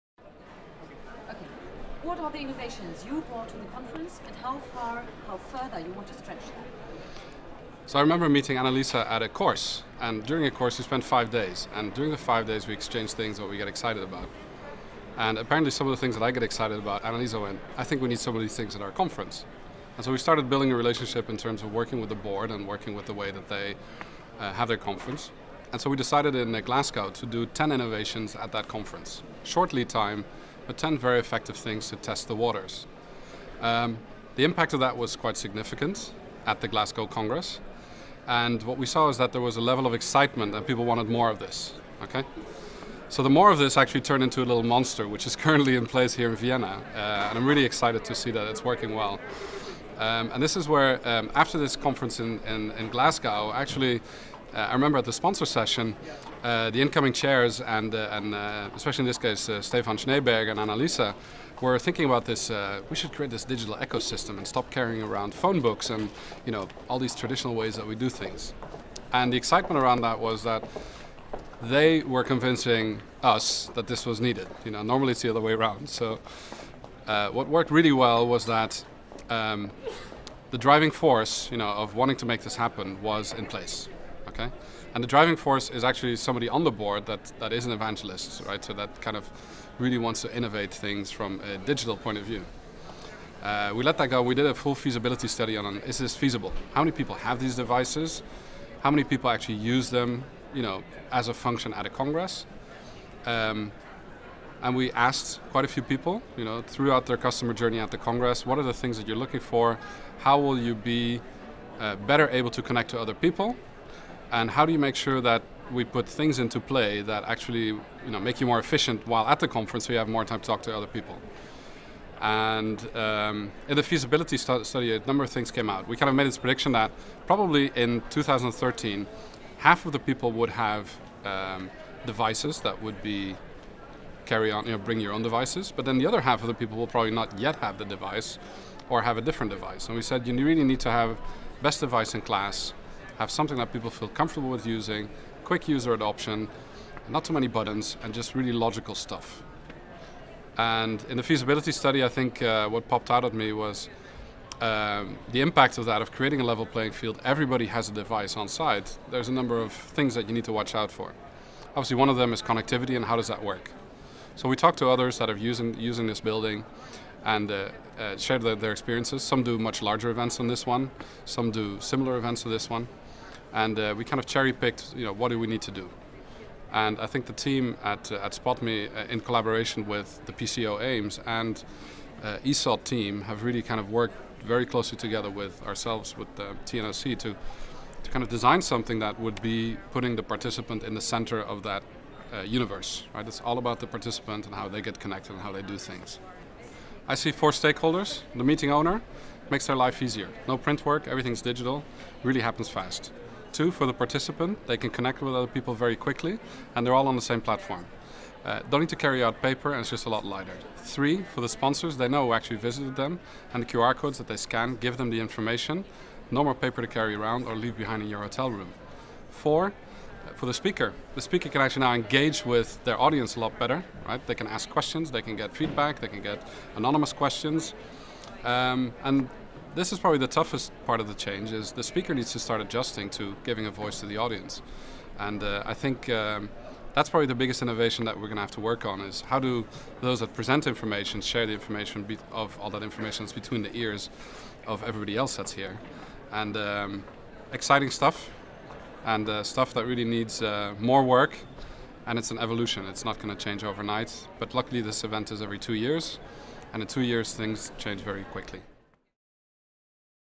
ESOTVienna2013 - "The impact of creating a level playing field"
Interview with Ruud Janssen, founder of The New Objective Collective